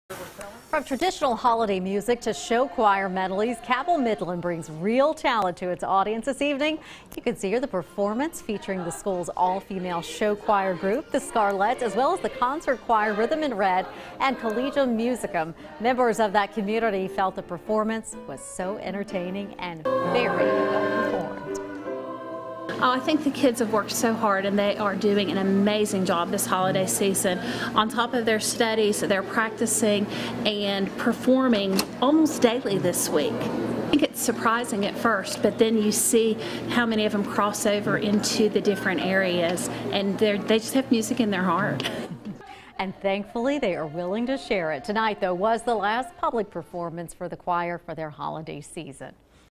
Cabell Midland Children's Concert Captivates
From traditional holiday music, to show choir, Cabell Midland brings real talent to its audience.